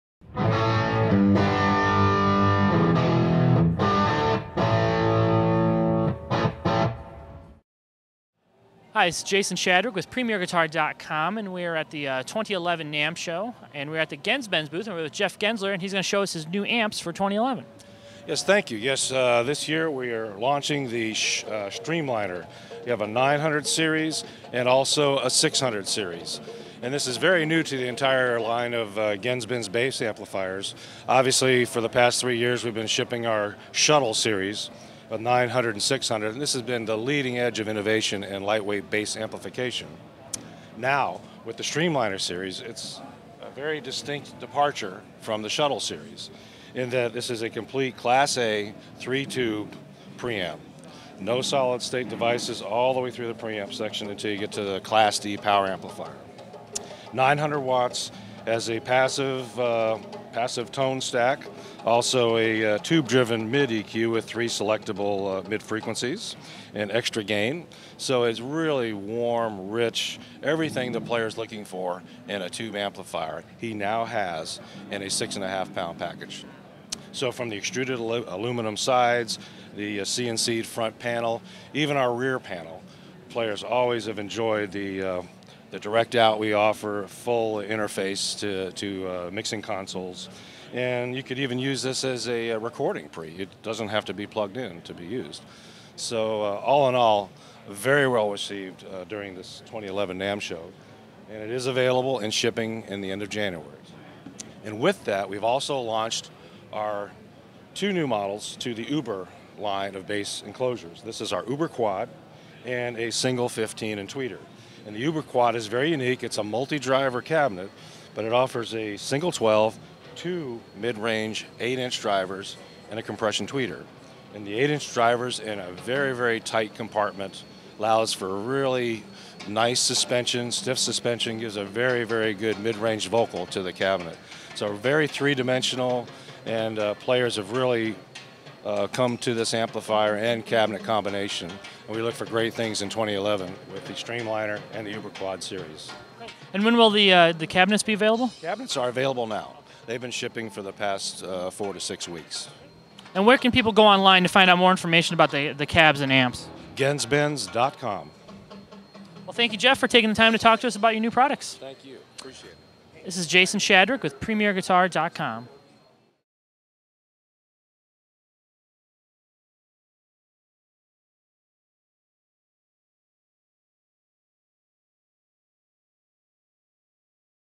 NAMM '11 - Genz Benz Streamliner Bass Series Amps & Uber Quad Bass Cabinets
PG's Jason Shadrick is On Location at the 2011 NAMM Show where he visits the Genz Benz booth. In this segment, we get to hear about Genz Benz's new Streamliner Bass Series Amps & Uber Quad Bass Cabinets.

For more 2011 NAMM videos, or to view Premier Guitar's completely FREE digital edition magazine be sure